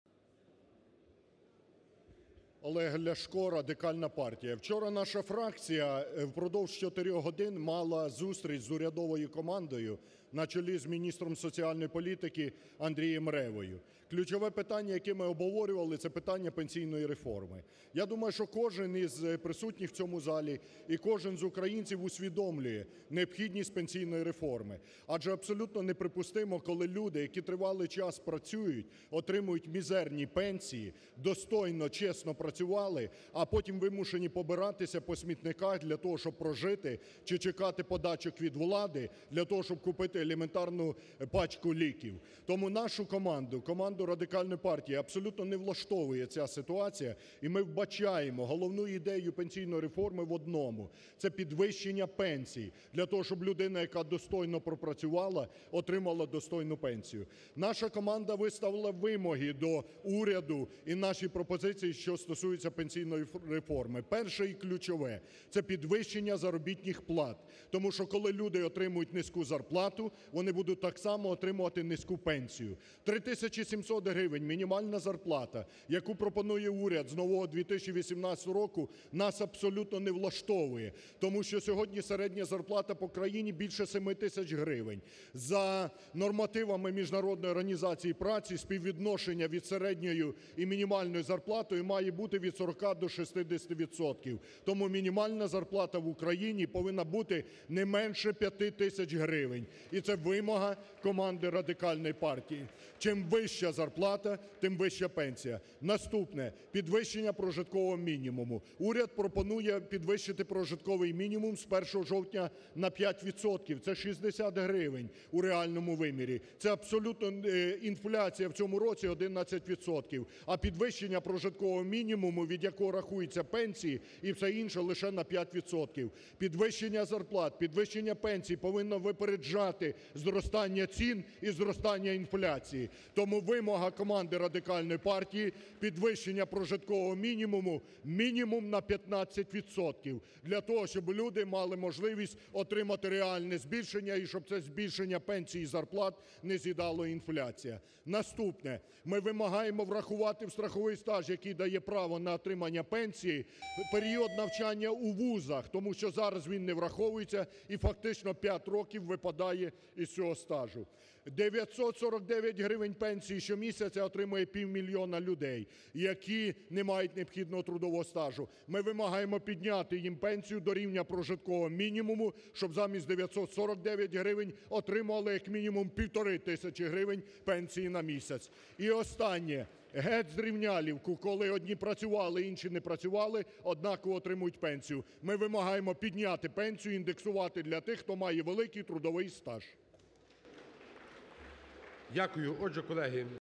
Ляшко: Темпи зростання пенсій та зарплат мають випереджати інфляцію!
Фракція Радикальної партії озвучила низку вимог до пенсійної реформи. За словами Олега Ляшка, головна мета пенсійної реформи – підвищення пенсій.
«Абсолютно неприпустимо, коли люди, які пропрацювали все життя, отримують мізерні пенсії та змушені побиратися по смітниках, щоб вижити. Або ж вони чекають подачок від влади, аби купити пачку необхідних ліків. Нашу команду ця ситуація категорично не влаштовує. Тому ми, як і кожен українець, усвідомлюємо необхідність впровадження пенсійної реформи», - заявив Ляшко.
Політик переконаний, що підвищити пенсії можливо лише за умови збільшення зарплат українців.
«Мінімальна зарплата у 3700 гривень, яку пропонує уряд з нового року, нас абсолютно не влаштовує. Сьогодні середня зарплата по України – 7000 гривень. Ми вимагаємо скоротити розрив між мінімальною та середньою зарплатою. Тому мінімальна зарплата в Україні має бути 5000 гривень, адже чим вища зарплата – тим більша пенсія. Це наша принципова позиція», - підкреслив лідер РПЛ.
Ще одна вимога радикалів – підвищення прожиткового мінімуму для пенсіонерів.
«Уряд пропонує з нового року підвищити прожитковий мінімум для пенсіонерів, від якого рахуються всі інші пенсії, на 5%. Це 60 гривень. Це абсолютно несправедливо, адже інфляція у цьому році складає 11%. Підвищення зарплат і пенсій повинно випереджати зростання цін. Тому вимога команди Радикальної партії – підвищення прожиткового мінімуму для пенсіонерів мінімум на 15%, щоб люди змогли відчути реальне зростання пенсій», - розповів Ляшко.
Серед інших, не менш важливих вимог РПЛ – включення навчання у вишах до страхового стажу, відновлення у повному обсязі пенсійних виплат для жінок, які вийшли на пенсію достроково, та індексація пенсій для людей з великим трудовим стажем і так званих соціальних пенсій.
«949 гривень на місяць сьогодні отримують півмільйона українців, які не мають необхідного трудового стажу. Ми вимагаємо підняти їм пенсію до рівня прожиткового мінімум для пенсіонерів, щоб вони замість 949 гривень отримували, як мінімум, 1500. Крім того, сьогодні люди, які працювали і не працювали, отримують однакові пенсії. Ми вимагаємо збільшити пенсію для людей, які мають великий трудовий стаж. Геть зрівнялівку!», - підсумував Ляшко.